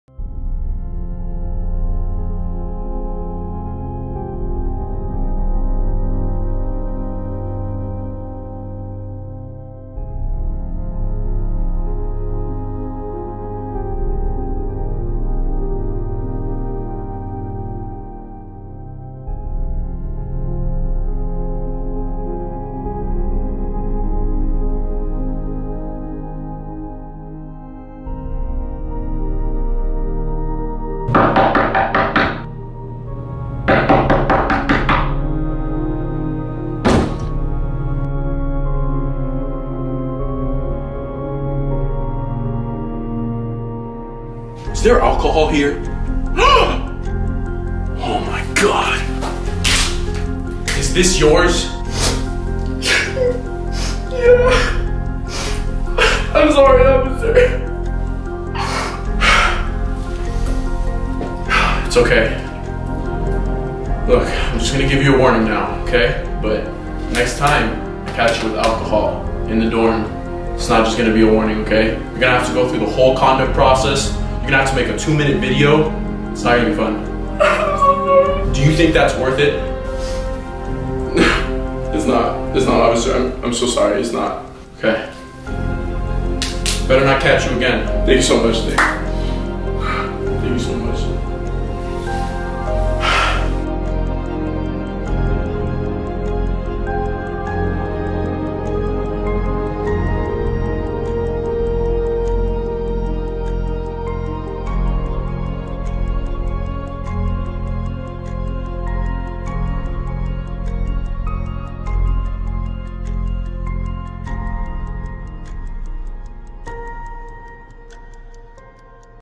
don't drink in dorms for office of student conduct
I had to make a video for the office of student conduct as one of my sanctions for being at a dorm party where there was alcohol

Add me on snapchat @drdoofuspants
Add me on instagram @doofdays
Add me on twitter @drdoofuspants